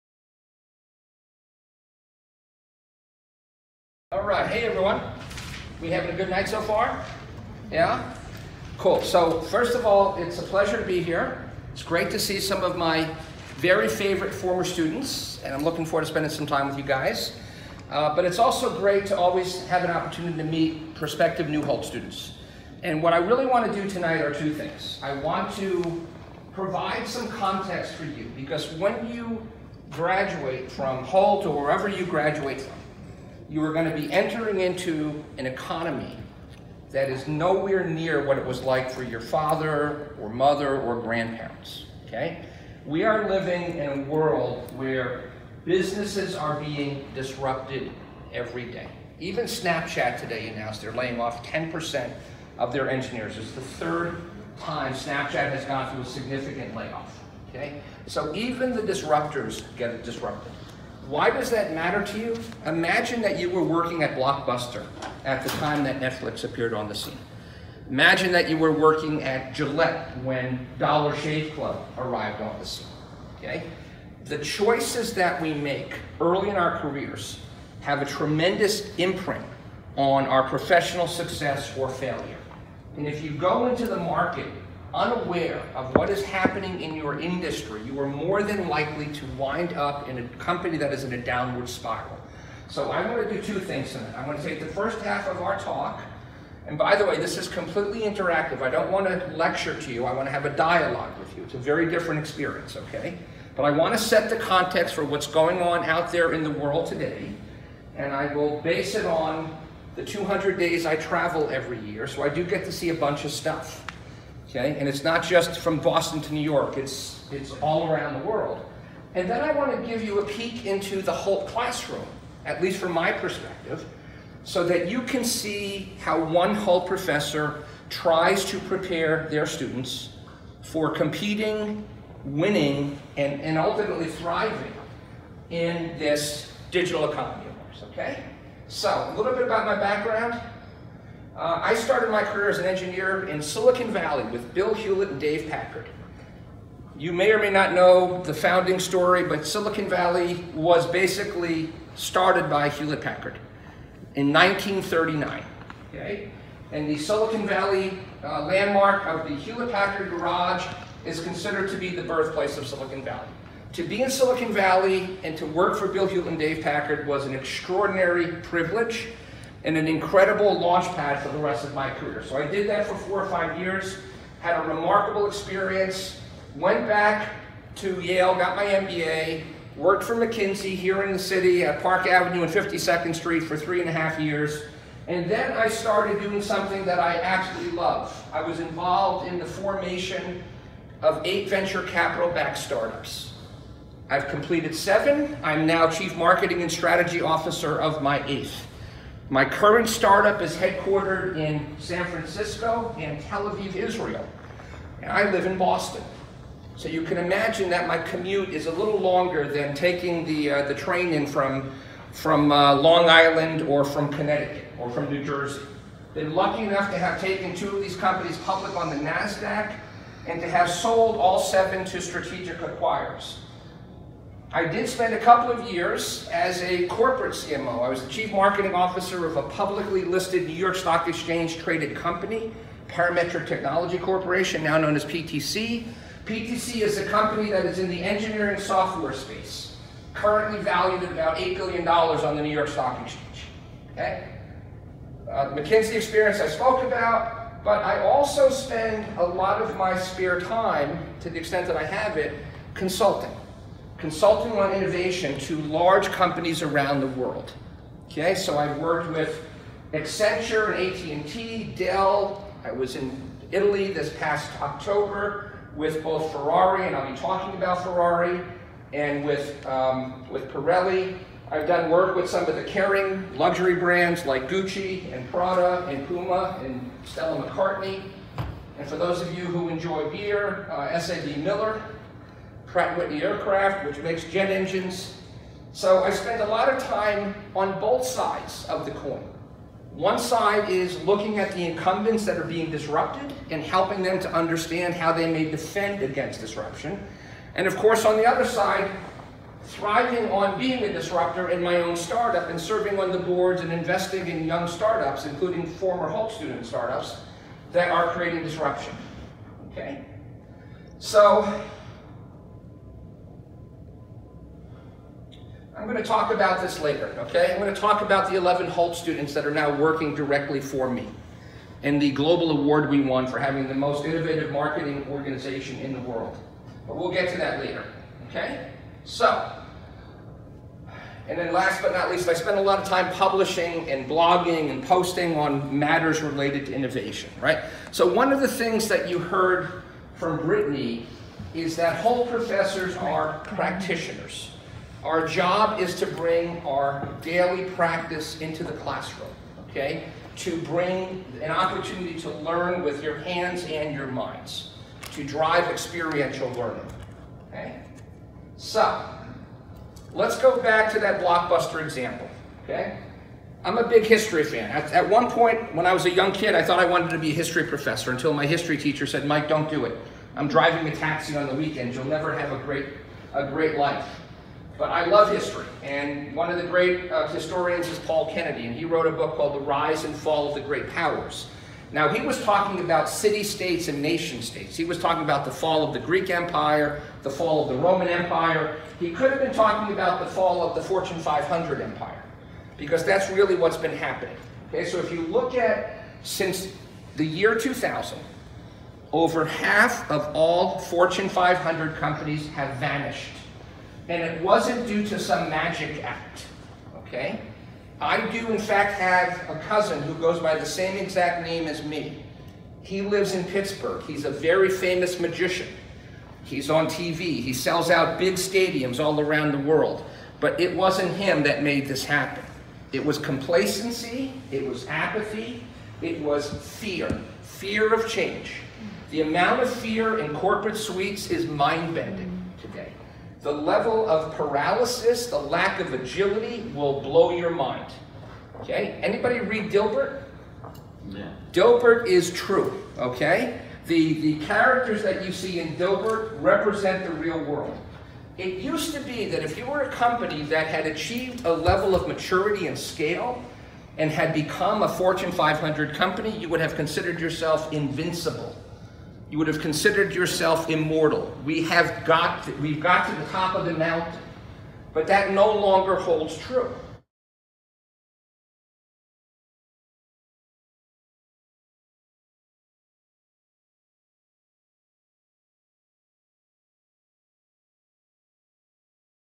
Disruptive Innovation MasterClass NYC March 2018
Professor Mike Grandinetti, serial entrepreneur & former McKinsey consultant, sets the tone for his talk on why the Global Generation needs to be deeply informed about the implications of Disruptive Innovation on their career and industry choices in today's permanent Fast Forward Digital Economy